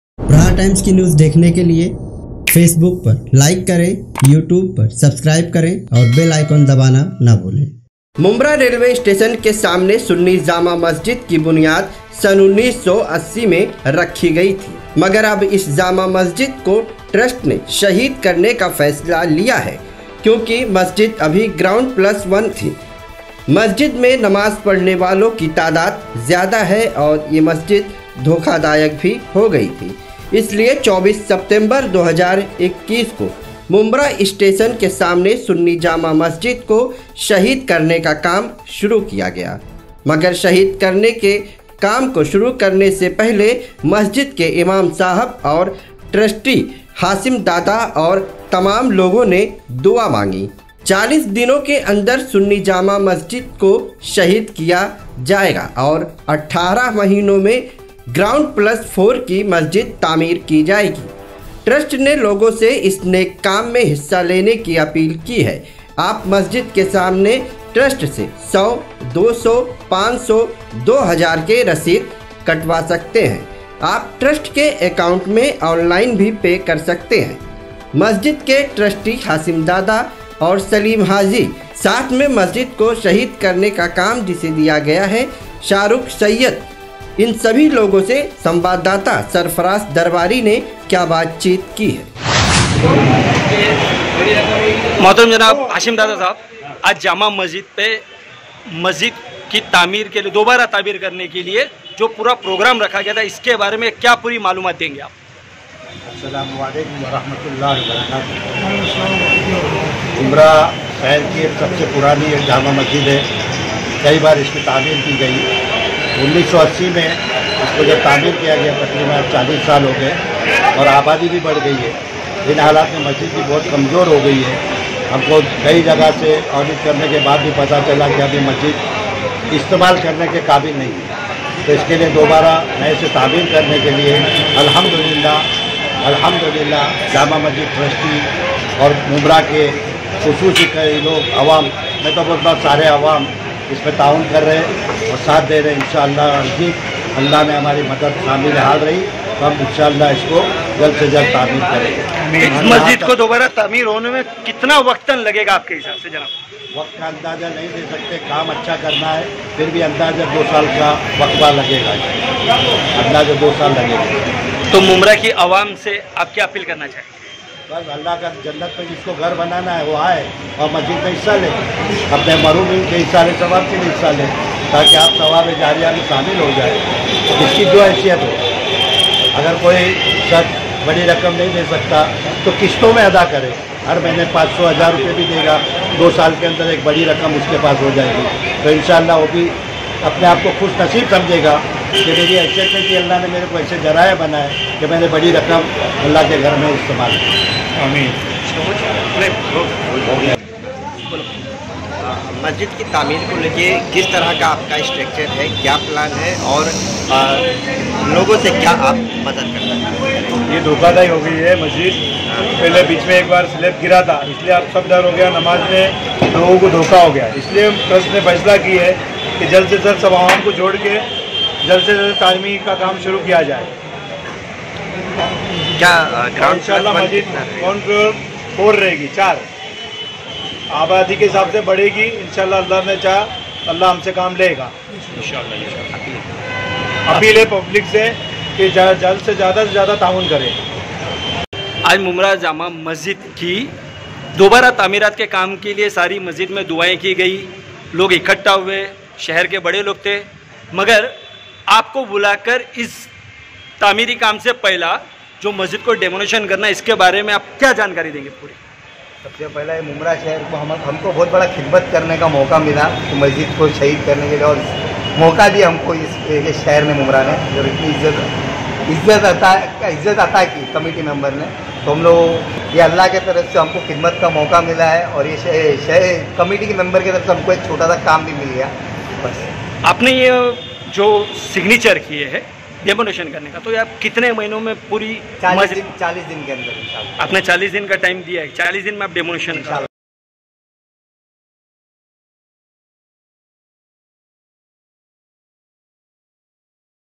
पुरानी सुन्नी जामा मस्जिद को G+4 बनाने की तैयारी शुरू || Islamuddin Khan || Aapka Prahar Times
पुरानी सुन्नी जामा मस्जिद को G+4 बनाने की तैयारी शुरू || Islamuddin Khan

मुम्ब्रा रेलवे स्टेशन के सामने पुरानी सुन्नी जामा मस्जिद को शहीद किया जाएगा और फिर से G+4 की मस्जिद बनाई जाएगी,ट्रस्ट ने इस नेक काम ने लोगो से हिस्सा लेने की अपील की है